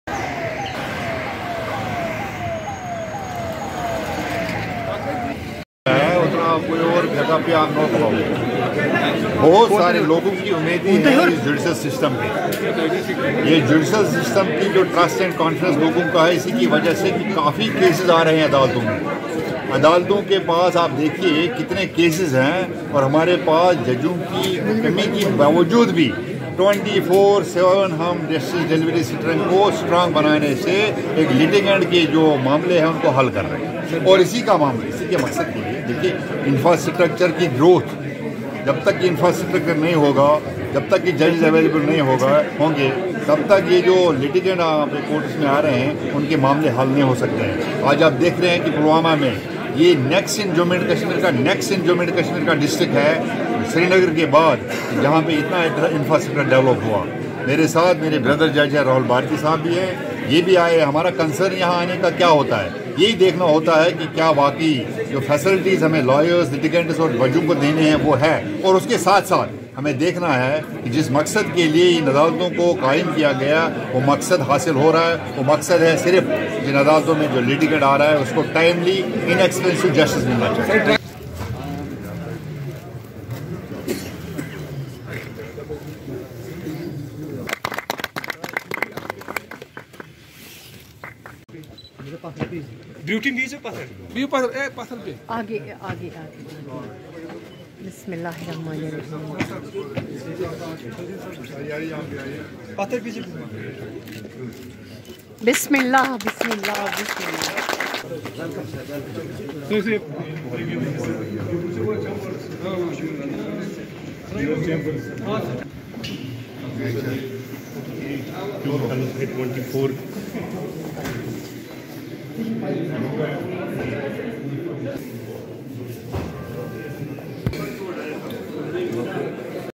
Justice Ali Mohammad Magrey, Judge J&K High Court, today visit of district court of Pulwama
Justice Ali Mohammad Magrey, Judge Jammu and Kashmir High Court,  today visit of district court of Pulwama.